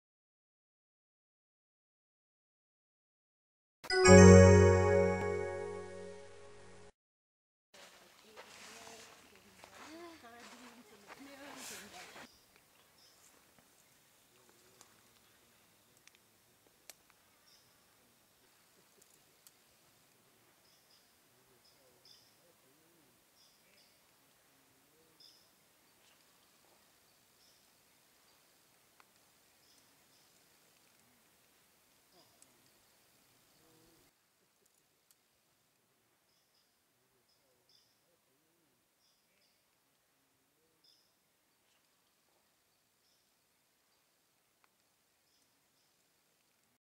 [호주_Australia] Lined Fire tailed Skink (줄무늬붉은꼬리도마뱀)
Class : Reptilia
Order : Chordata
Family : Scincidae
Genus : Morethia
Species name : Morethia ruficauda
English name : Lined Fire-tailed Skink
Korean name : 줄무늬붉은꼬리도마뱀
Observed location : North Australia